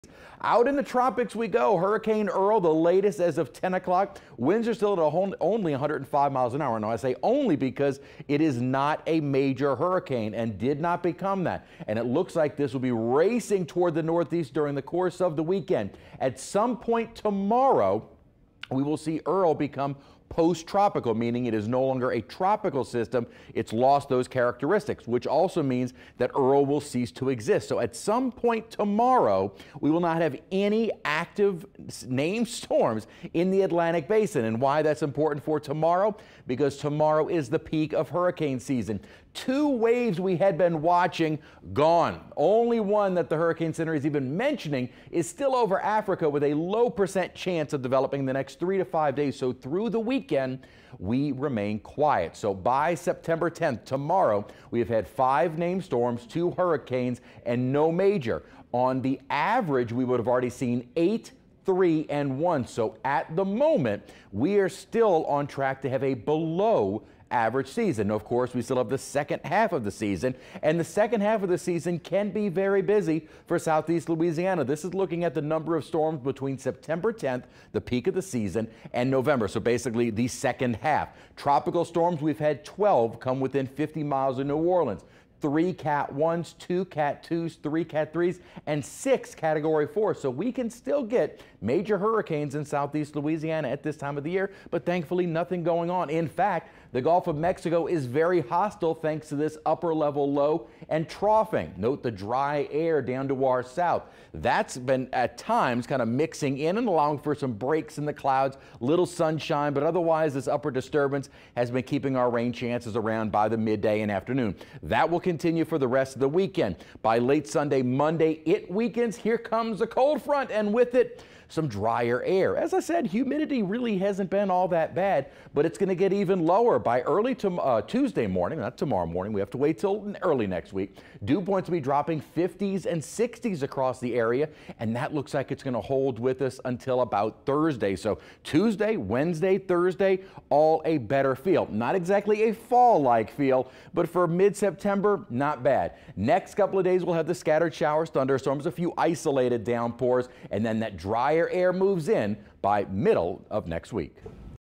Tropical Update: Hurricane Earl moving away from Bermuda
As Earl has failed to become a major hurricane as it moves away from Bermuda, the likelihood is very low. So we have not yet seen our first major hurricane of the season! That's fine by me!! Invest 95 which at one time was given a 70% high likelihood of development has been decreased to 10%. Dry air and wind shear have won out. And that appears to be the pattern for the next several days with Saharan dust over a few others areas the NHC is highlighting.

Once Earl is gone, it's possible we won't have an active storm in the Atlantic around the peak of the season. In fact, Saturday is the climatological peak of hurricane season. In an average season, by September 10th, we would have seen 8 named storms, 3 hurricanes, and 1 major. This year continues to run below average with 5 named and 2 hurricanes.

Of course, we are only at the top of the mountain and late September and early October are still very much the heart of the season. Models continue to keep us quiet through next week. Let's keep the strong cold fronts coming to block the tropics!